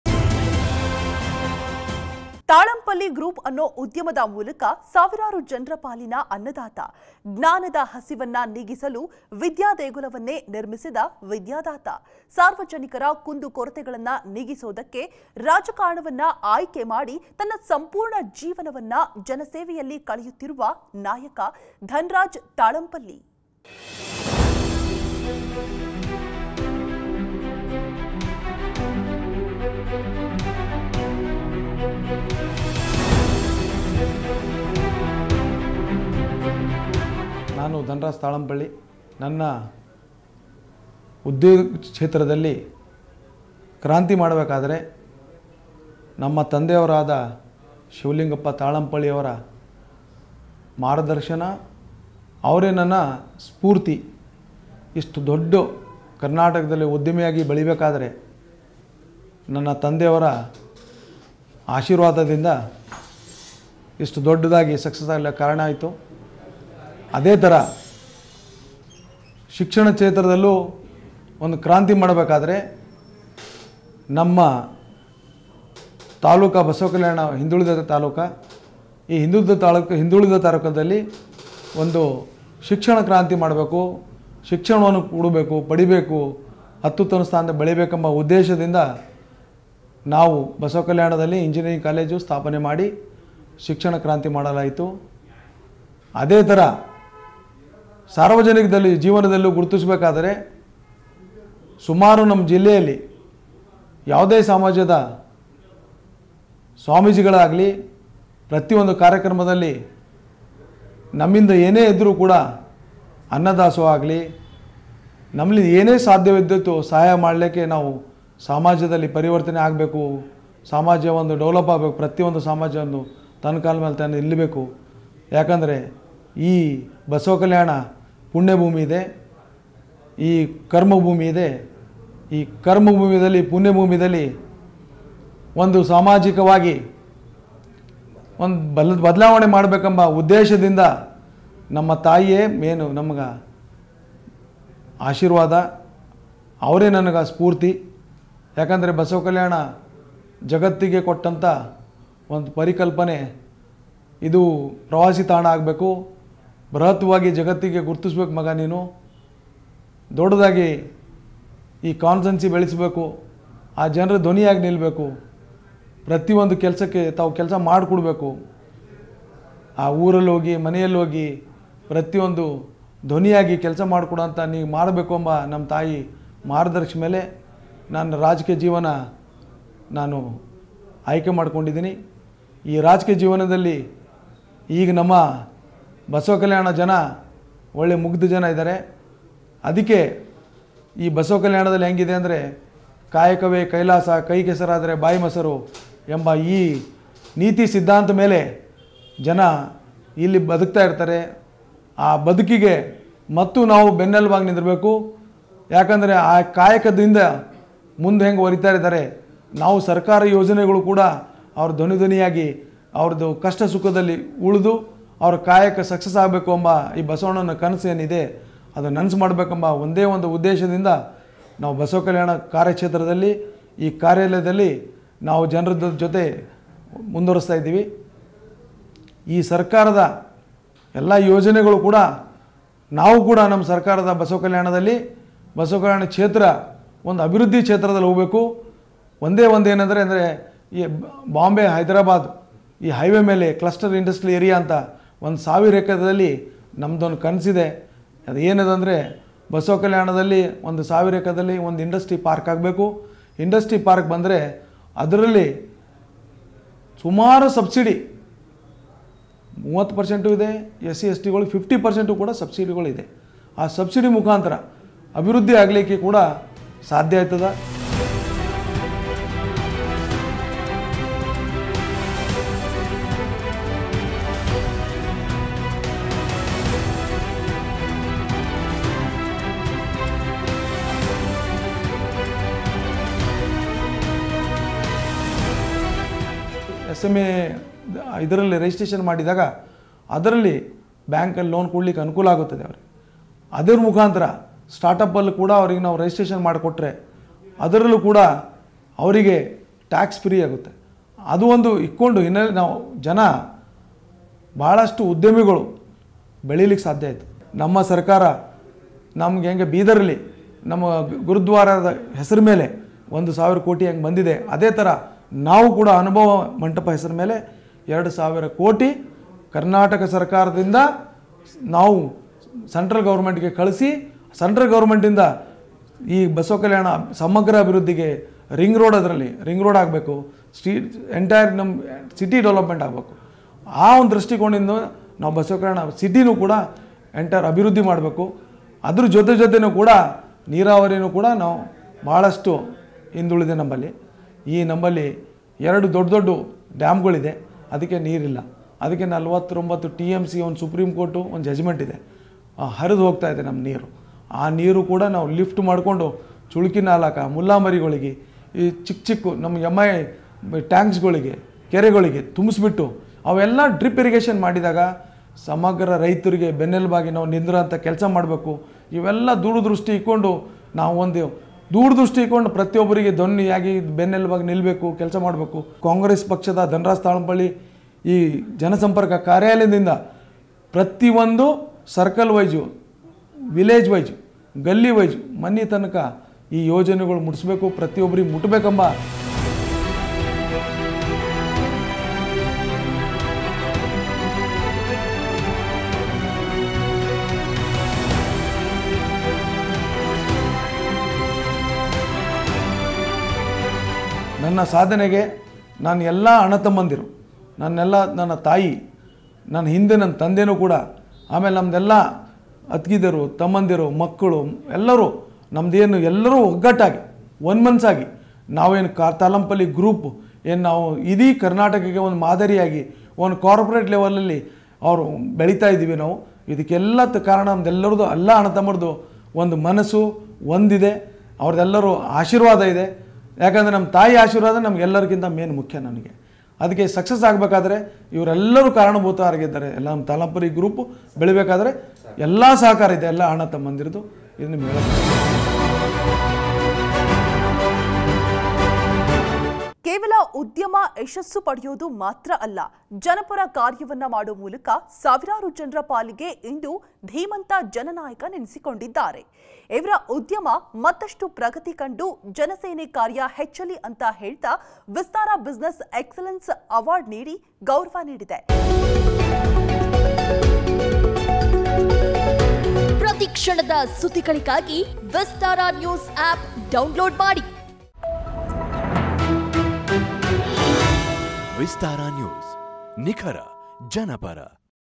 ಬಸವಕಲ್ಯಾಣದ ಅಭಿವೃದ್ಧಿಗೆ ಪಣ ತೊಟ್ಟಿರುವ ನಾಯಕ.! | Dhanraj Talampally Basavakalyan |Vistara Business Award
Explore the fascinating story of Dhanraj Talampally Basavakalyan in this exclusive video from Vistara Business And Property! 🎥🔍 Dive into the history, culture, and significance of this remarkable location in Karnataka, India. 🇮🇳🌟

Stay updated with the latest news, politics, current affairs, sports, business, and cinema from Karnataka and around the world. 👍😊